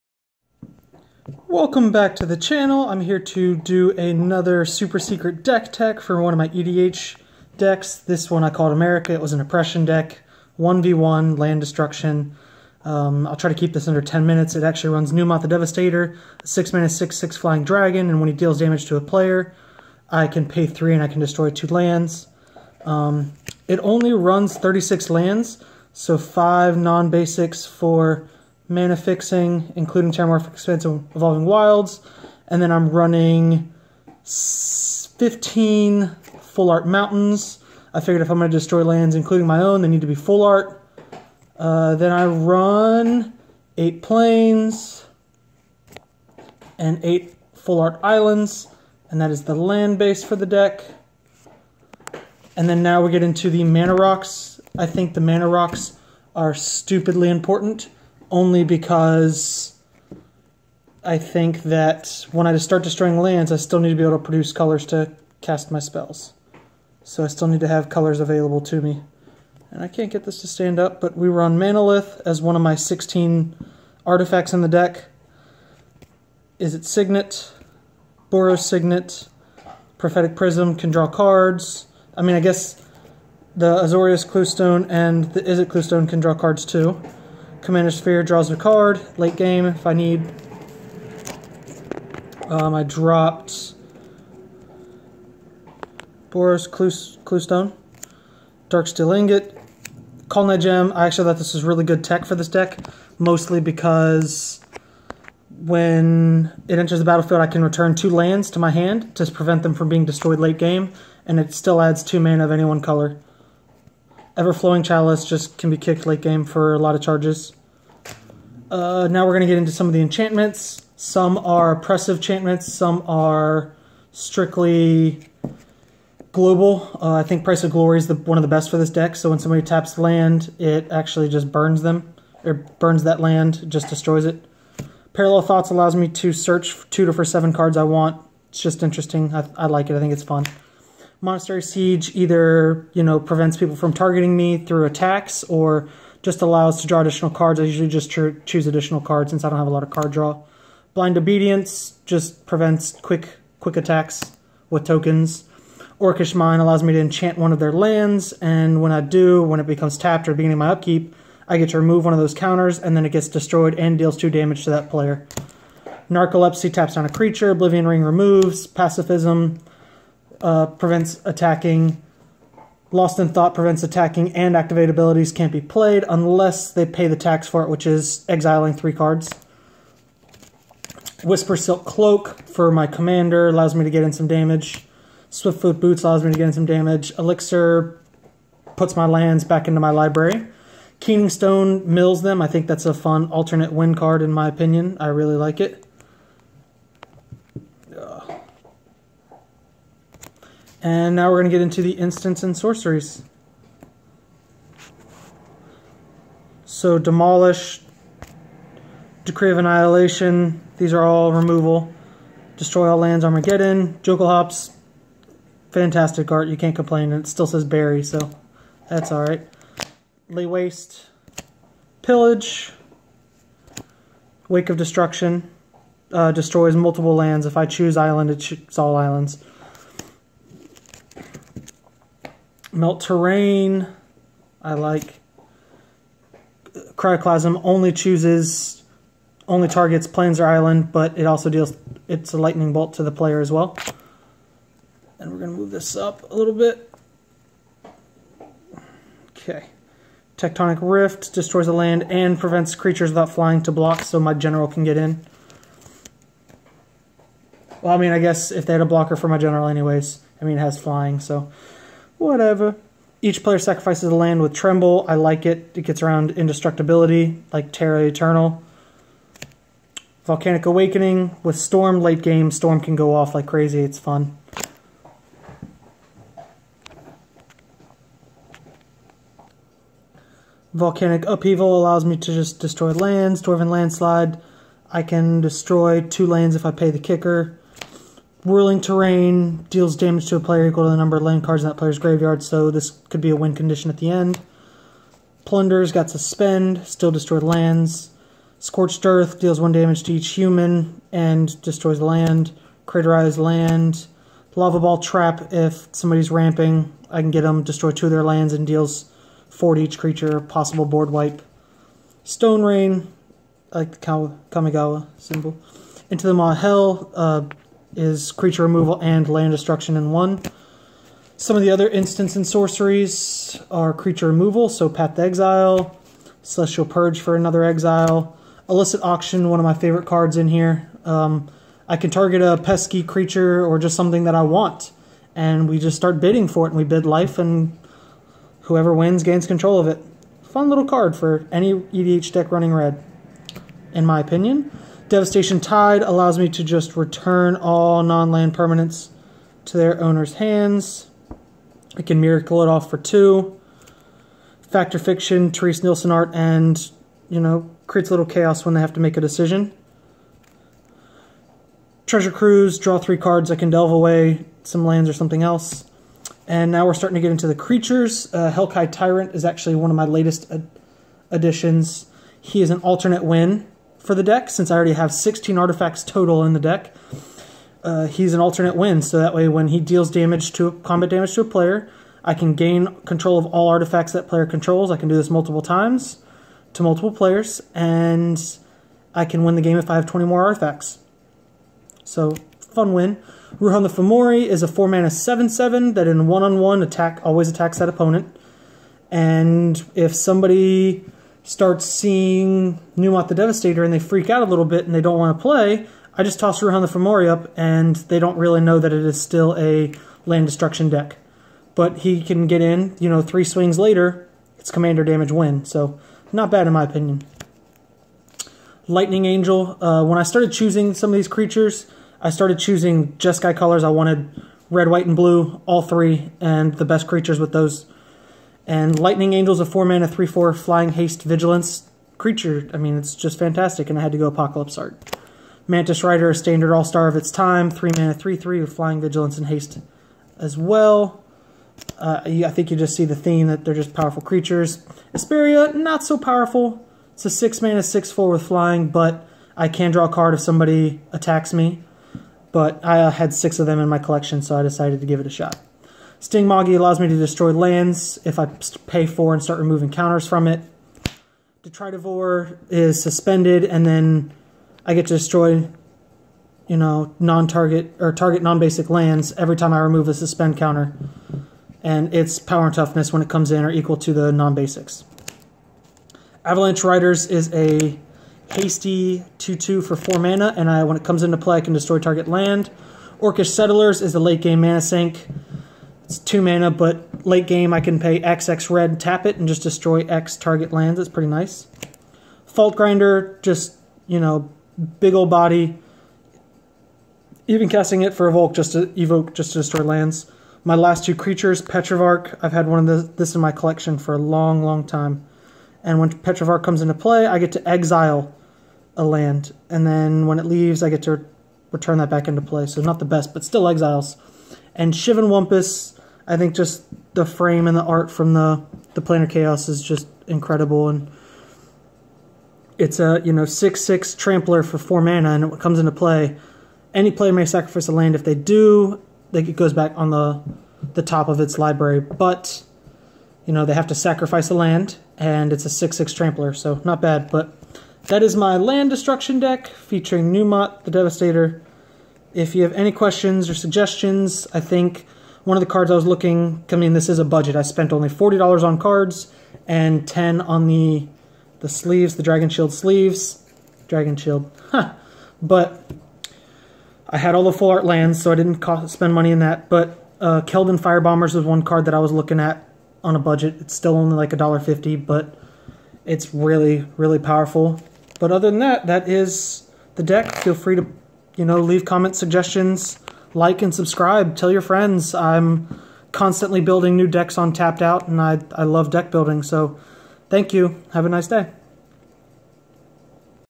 Numot, The Devastator: Budget EDH/Commander Deck Tech - American Oppression
Jeskai Land Destruction duel commander deck.  Built on a $50 budget for cards and Dragonshield Golds.